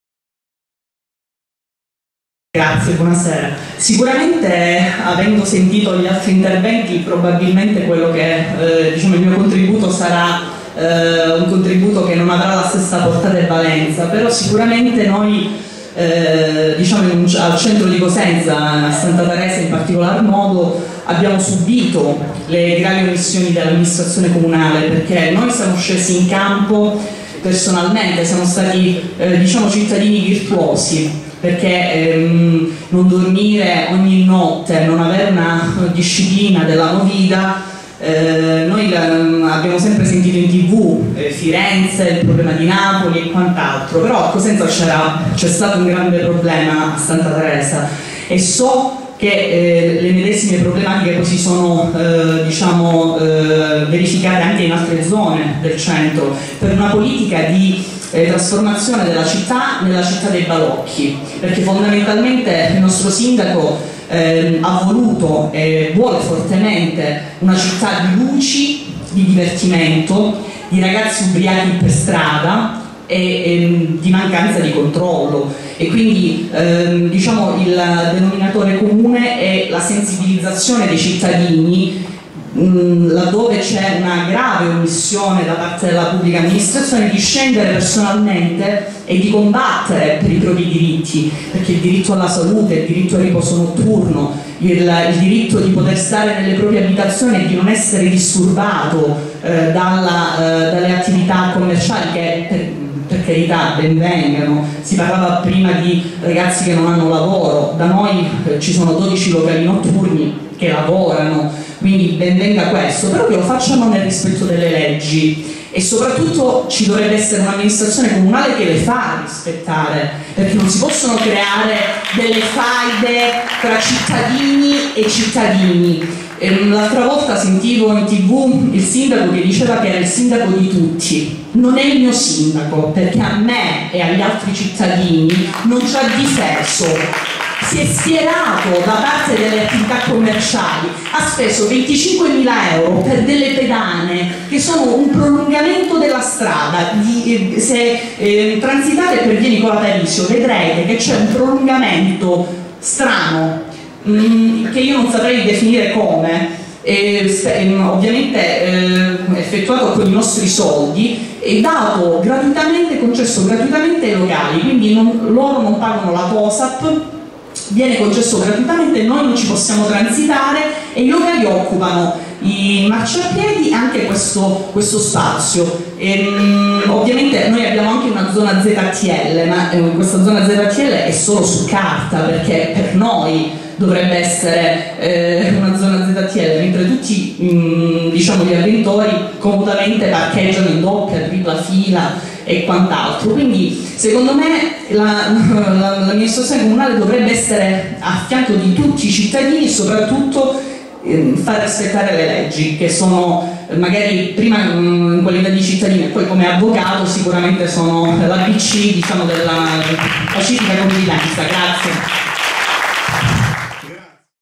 "Cosenza Città Aperta" a cura del Movimento NOI: Gabriella Marini Serra
“Cosenza Città Aperta” è la prima edizione del confronto pubblico voluto dalla Comunità Politica del Movimento Cattolico NOI Rete Umana che, con la sua Politica innovativa è progettualmente concreta sta attraendo tutti coloro i quali vogliono vedere risorgere le meravigliose Città Italiane, in questo caso la Città Storica di Cosenza.

L'intervento dell'Avv. Gabriella Marini Serra di Cosenza.

Canale You Tube ufficiale del Movimento NOI Rete Umana